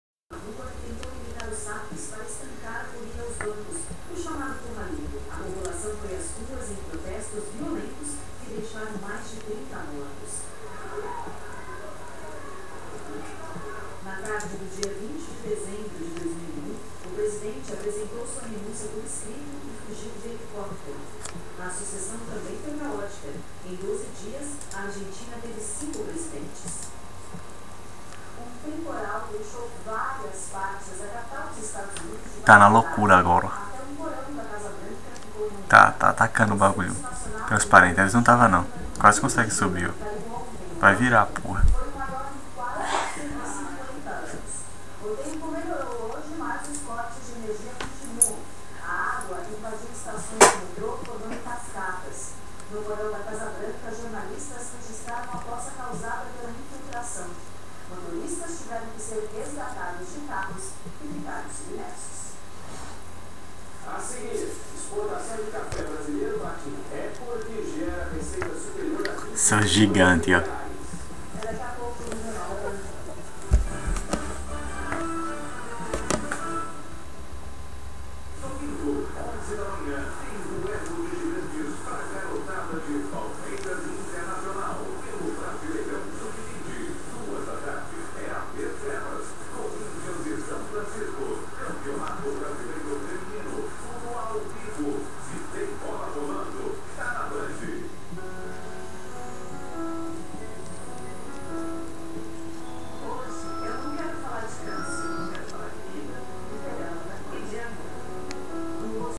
Dinoponera presa antes de ser dissecada
Ela estava fazendo movimentos interessantes; pondo seu metasome mais elevando que o resto de seu corpo e a cabeça mais baixo - como aracnídeos ameaçando.
Assim como ela destroçou a menor e arrancou apenas o conteúdo de dentro deixando o exoesqueleto.
Contudo tudo que o vídeo mostra é quando ela já está entediada e começa à rodar contra o plástico.